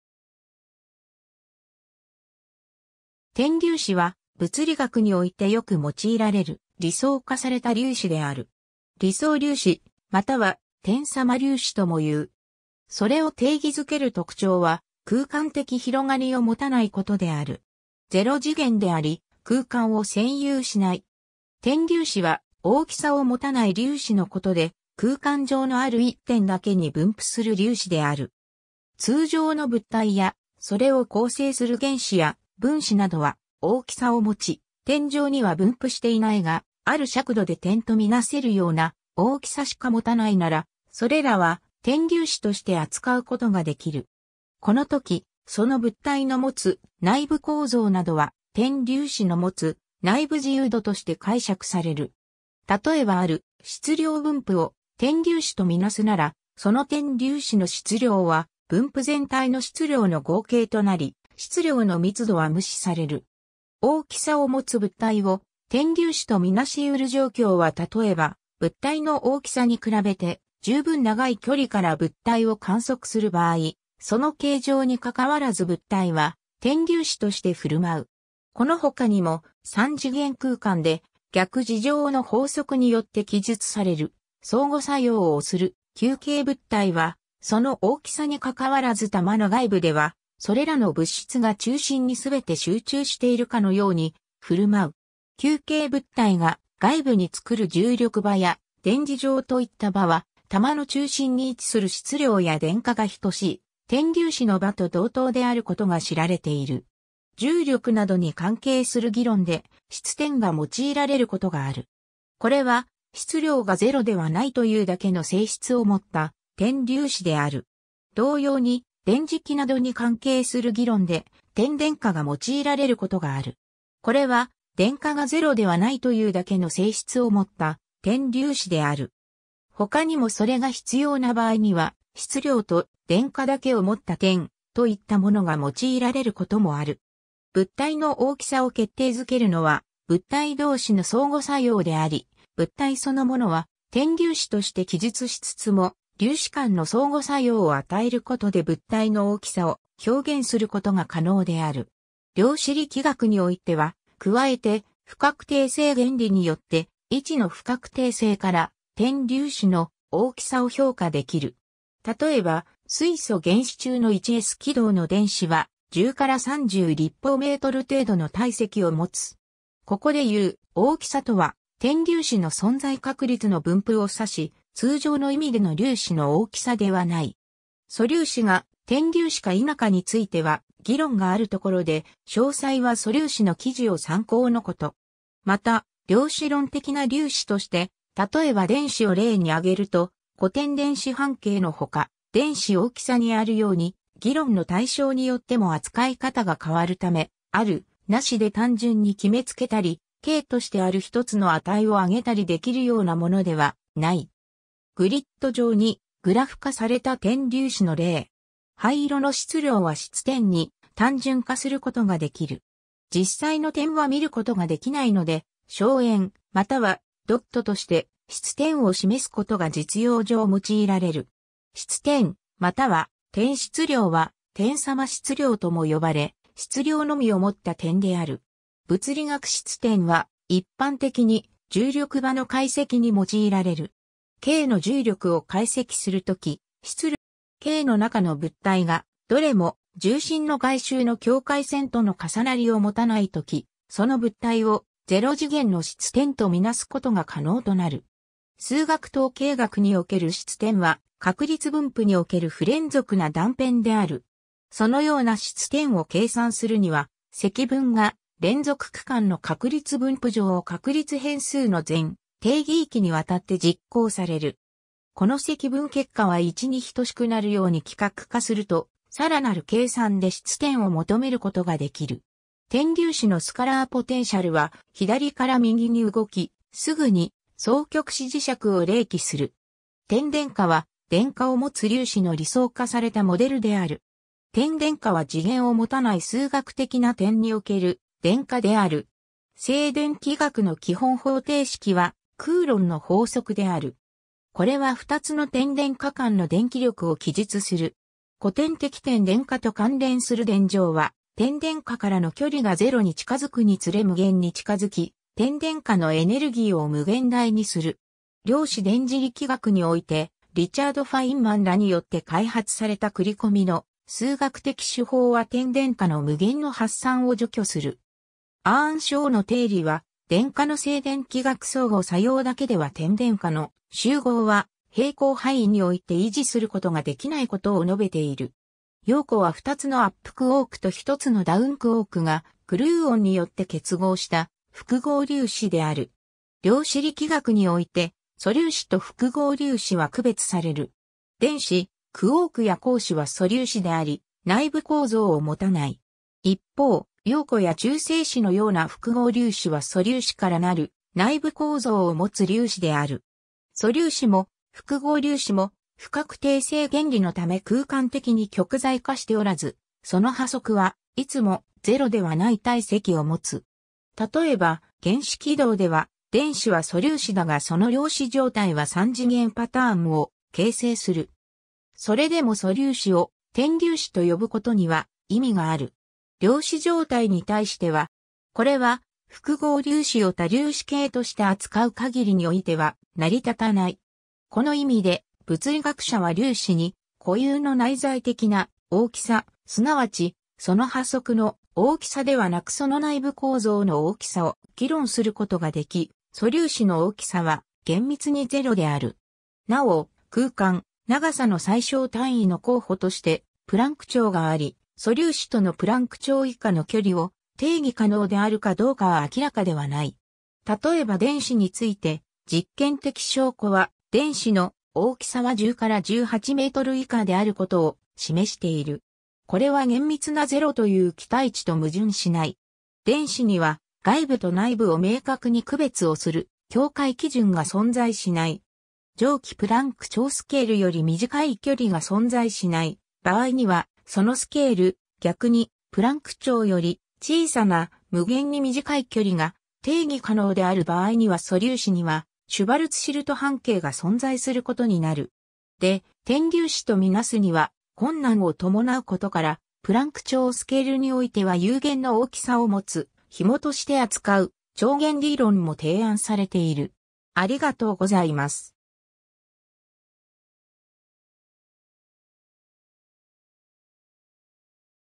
点粒子
点粒子（英: point particle）は、物理学においてよく用いられる理想化された粒子である。
理想粒子 (ideal particle) または点様粒子 (point-like particle, pointlike—) とも言う。
 それを定義付ける特徴は空間的広がりを持たないことである。
ゼロ次元であり空間を占有しない。
 点粒子は大きさを持たない粒子のことで、空間上のある一点だけに分布する粒子である。
通常の物体やそれを構成する原子や分子などは大きさを持ち、点状には分布していないが、ある尺度で点と見なせるような大きさしか持たないなら、それらは点粒子として扱うことができる。
このとき、その物体の持つ内部構造などは点粒子の持つ内部自由度として解釈される。
たとえばある質量分布を点粒子と見なすなら、その点粒子の質量は分布全体の質量の合計となり、質量の密度は無視される。
 大きさを持つ物体を点粒子と見なし得る状況は例えば、物体の大きさに比べて充分長い距離から物体を観測する場合、その形状に関わらず物体は点粒子として振る舞う。
この他にも、三次元空間で逆二乗の法則によって記述される相互作用をする球形物体は、その大きさにかかわらず球の外部では、それらの物質が中心に全て集中しているかのように振る舞う。
球形物体が外部に作る重力場や電磁場といった場は、球の中心に位置する質量や電荷が等しい点粒子の場と同等であることが知られている。
 重力などに関係する議論で、質点 (point mass) が用いられることがある。
これは質量（マス）がゼロではないというだけの性質を持った点粒子である。
同様に電磁気などに関係する議論で、点電荷 (point charge) が用いられることがある。
これは電荷（チャージ）がゼロではないというだけの性質を持った点粒子である。
他にもそれが必要な場合には、質量と電荷だけを持った点、といったものが用いられることもある。
 物体の大きさを決定づけるのは物体同士の相互作用であり、物体そのものは点粒子として記述しつつも、粒子間の相互作用を与えることで物体の大きさを表現することが可能である。
 量子力学においては、加えて不確定性原理によって、位置の不確定性から点粒子の「大きさ」を評価できる。
例えば、水素原子中の 1s 軌道の電子は、10-30 m3 程度の体積を持つ。
ここでいう「大きさ」とは点粒子の存在確率の分布を指し、通常の意味での粒子の大きさではない（点粒子の大きさはゼロである）。
 素粒子が点粒子か否かについては議論がある所で、詳細は素粒子の記事を参考のこと。
また、量子論的な粒子として、例えば電子を例に挙げると、古典電子半径の他、電子#大きさにあるように、議論の対象によっても扱い方が変わるため、「ある / なし」で単純に決めつけたり、径としてある1つの値を挙げたりできるようなものではない。
 グリッド上にグラフ化された点粒子の例。
灰色の質量は質点（黒い円）に単純化することができる。
実際の点は見ることができないので、小円またはドットとして質点を示すことが実用上用いられる。
 質点（しつてん、英: point mass）または点質量（てんしつりょう）は、点様質量（てんようしつりょう、英: pointlike mass）とも呼ばれ、質量のみを持った点である。
 物理学 質点は一般的に重力場の解析に用いられる。
系の重力を解析するとき、質量の全単位を個別に計上することは不可能となる。
系の中の物体がどれも重心の外周の境界線との重なりを持たないとき、その物体をゼロ次元の質点と見なすことが可能となる。
 数学 統計学における質点は確率分布における不連続な断片である。
そのような質点を計算するには積分が連続区間の確率分布上を確率変数の全定義域に渡って実行される。
この積分結果は1に等しくなるように規格化すると、さらなる計算で質点を求めることができる。
 点粒子のスカラーポテンシャルは左から右に動き、すぐに双極子磁石を励起する。
 点電荷 (point charge) は電荷を持つ粒子の理想化されたモデルである。
点電荷は次元を持たない数学的な点における電荷である。
 静電気学の基本方程式はクーロンの法則である。
これは二つの点電荷間の電気力を記述する。
古典的点電荷と関連する電場は、点電荷からの距離がゼロに近づくにつれ無限に近づき、点電荷のエネルギー（質量）を無限大にする。
量子電磁力学において、リチャード・フ...